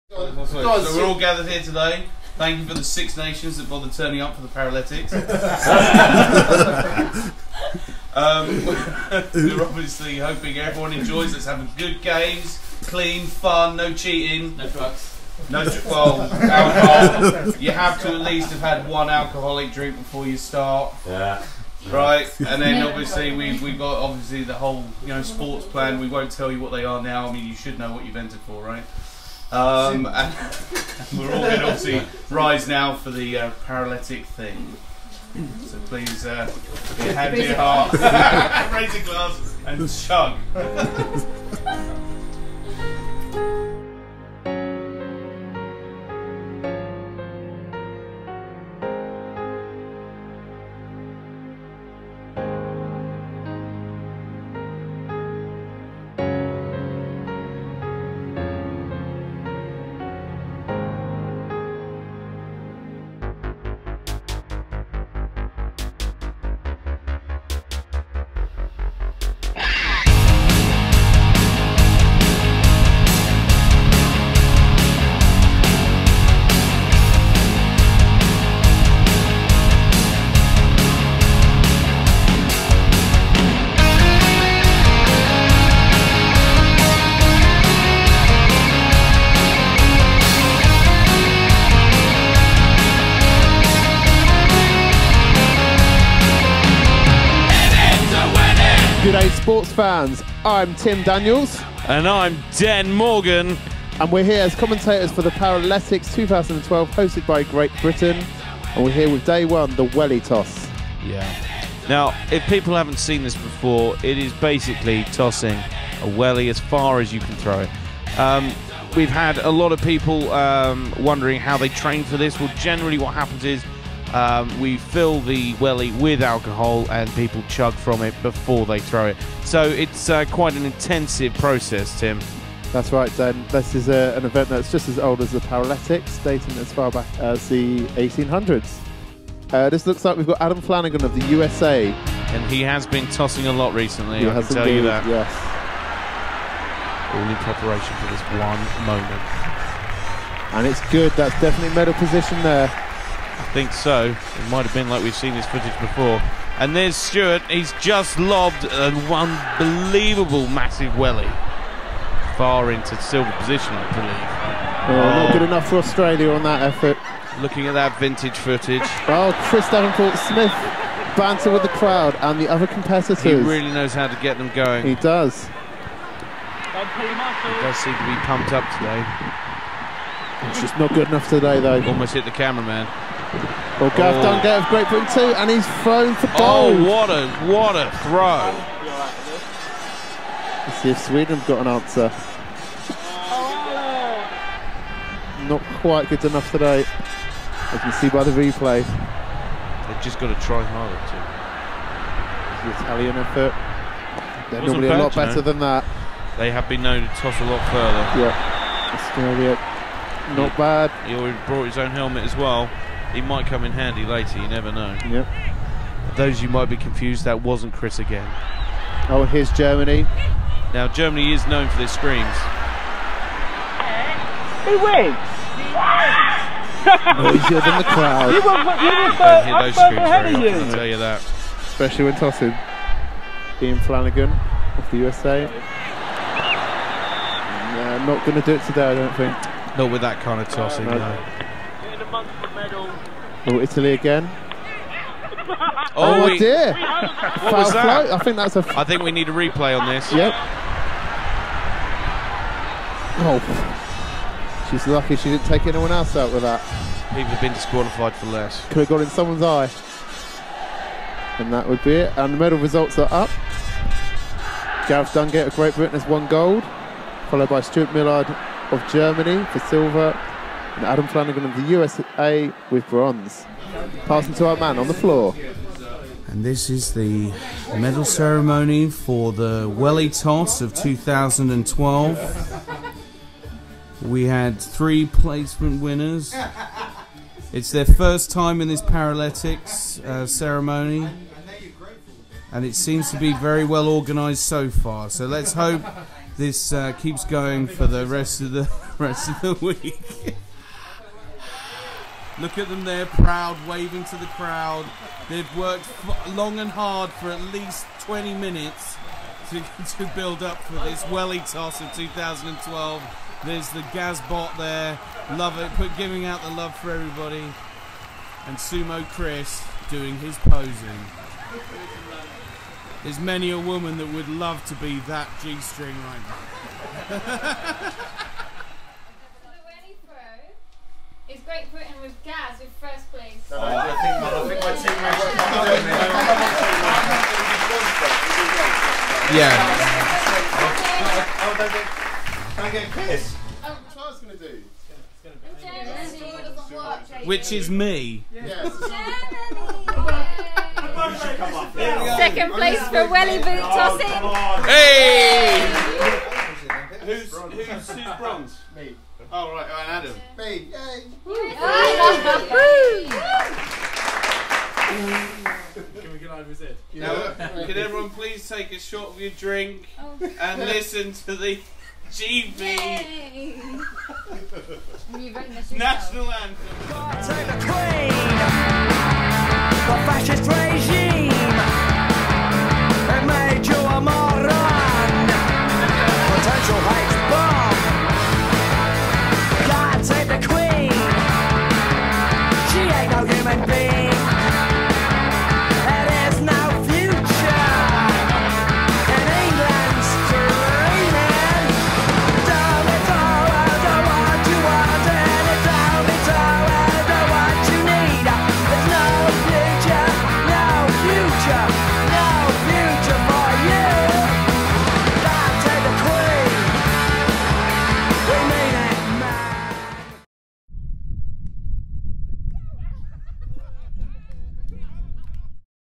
Paralytics 2012 - Intro & Welly Toss Pt.1
For my birthday we had our own Olympics/sports themed party dubbed the Paralytics with numerous events and countries involved. In Part 1, we have the opening of the games as well as the Welly Toss event. Enjoy!